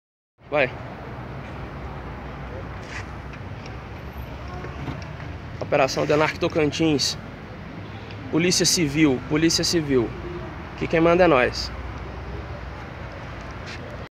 Operação TRASGO da Denarc apreende 88kg de maconha
Trio estaria vindo de Goiânia com drogas destinadas à distribuição em Palmas e região. Operação Trasgo mira na atuação do Cartel Goiano no Tocantins.